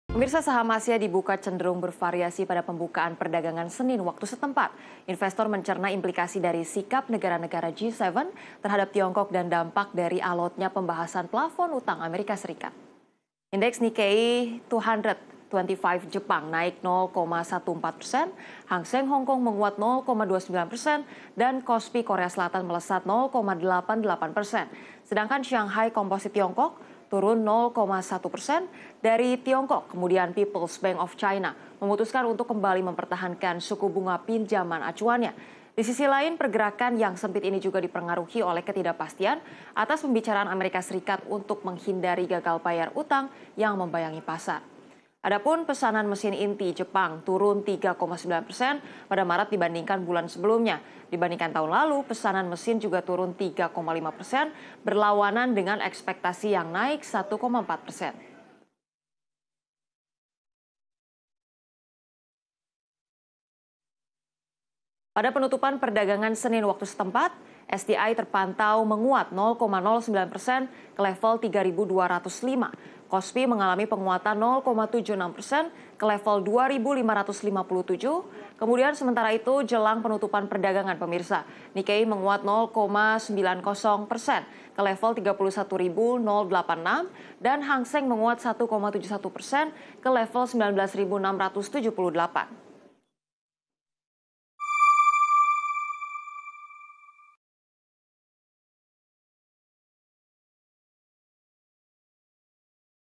Intip! Pergerakan Bursa Asia di Berbagai Indeks | IDX CHANNEL
Saham Asia dibuka cenderung bervariasi, pada pembukaan perdagangan Senin (22/05) waktu setempat. Investor mencerna implikasi, dari sikap negara-negara G7 terhadap China, dan dampak dari alotnya pembahasan plafon utang Amerika Serikat.